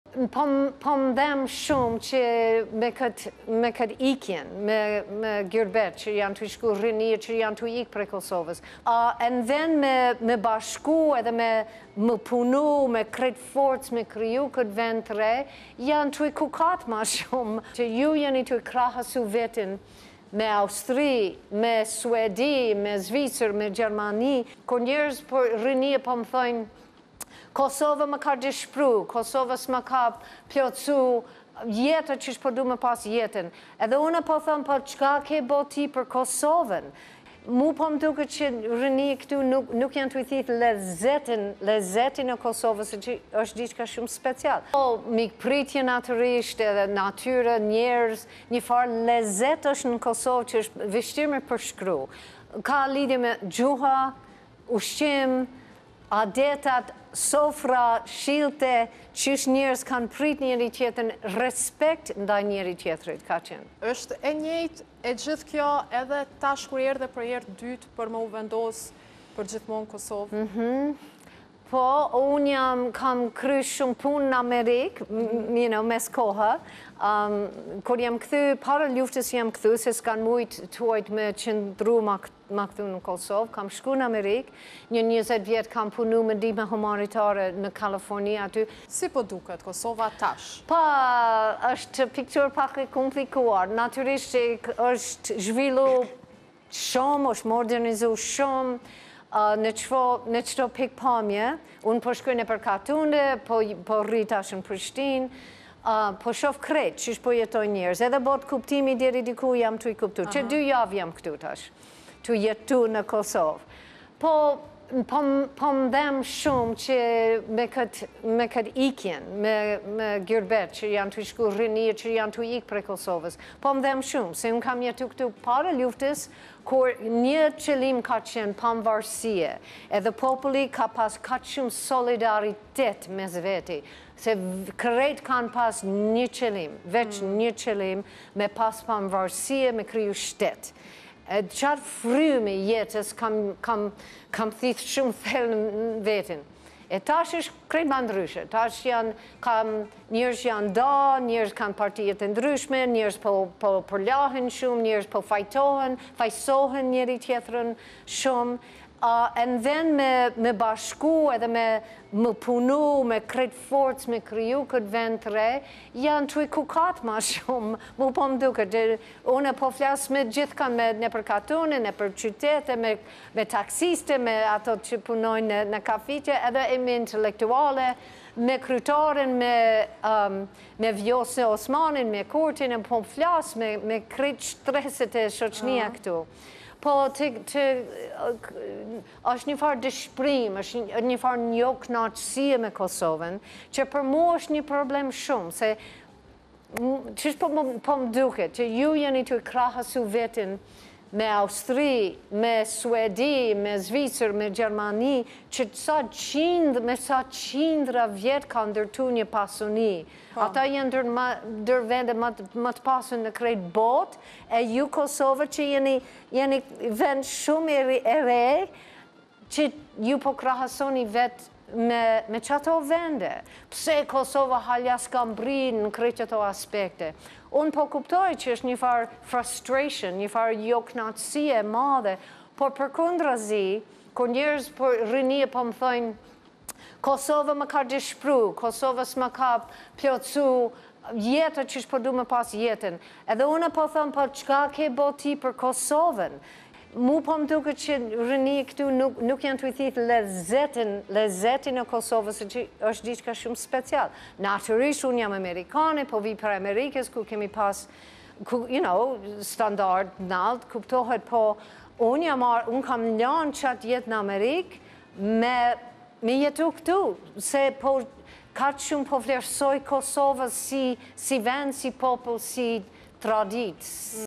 "Po kukasin me shume, nuk po ia ndijne Kosoves lezetin", amerikania qe jeton ne Kosove...
"Po kukasin me shume, nuk po ia ndijne Kosoves lezetin", amerikania qe jeton ne Kosove: Po krahasoheni me Austrine e Suedine